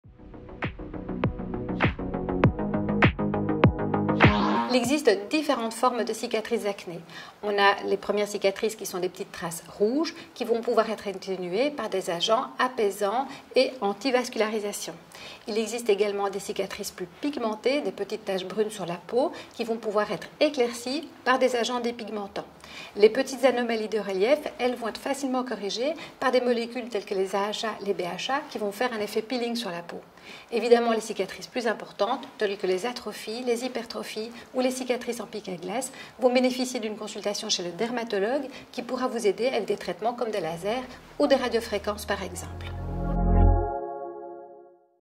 SANOFLORE | Comment se débarrasser des cicatrices d'acné ?
Première cause de consultation chez le dermatologue, l’acné est une maladie inflammatoire du follicule pileux qui touche près de 6 millions de personnes en France, majoritairement des adolescents mais aussi des adultes.  Les boutons d’acné peuvent laisser des cicatrices plus ou moins marquées (et marquantes). Alors comment s'en débarrasser ? Dr Braeken, dermatologue conseil, vous répond en vidéo !